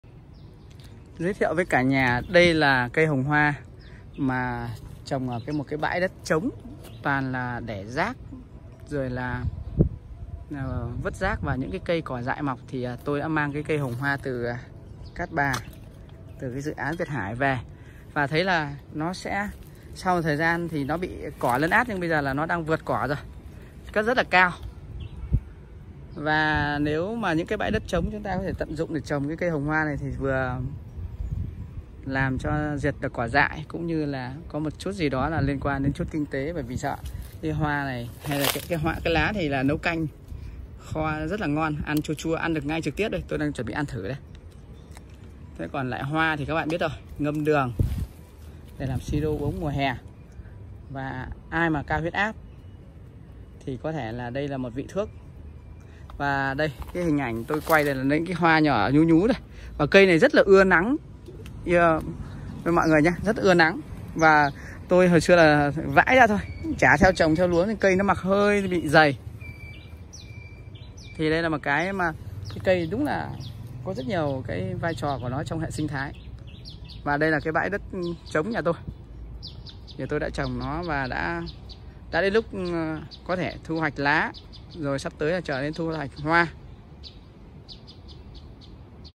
Cây gì mà lá ăn được, hoa ngâm đường, ngâm rượu và sức sống mãnh liệt
Tựa đề đã nói lên tất cả